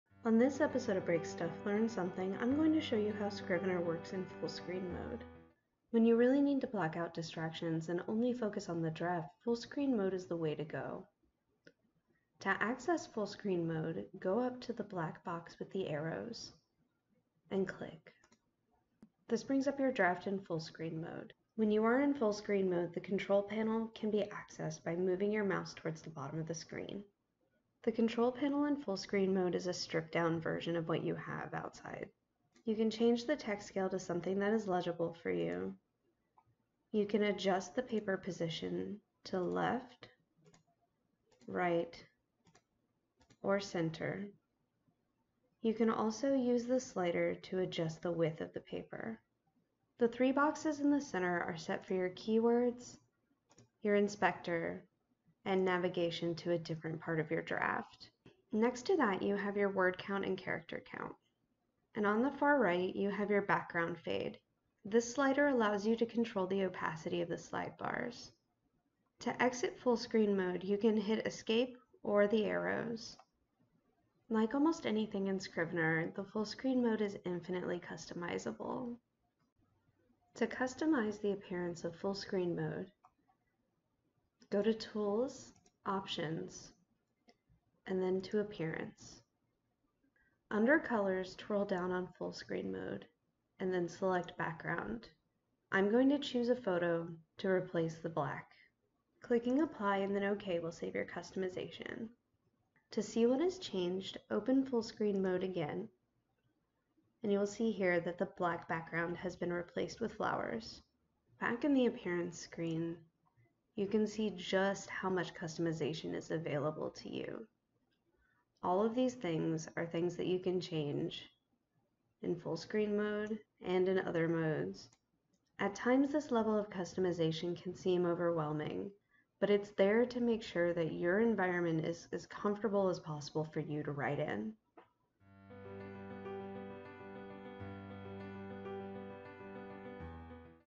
Scrivener Full Screen Mode and Customization
This tutorial shows beginners how to use and customize the full screen mode in Scrivener.

Photo Credit to the author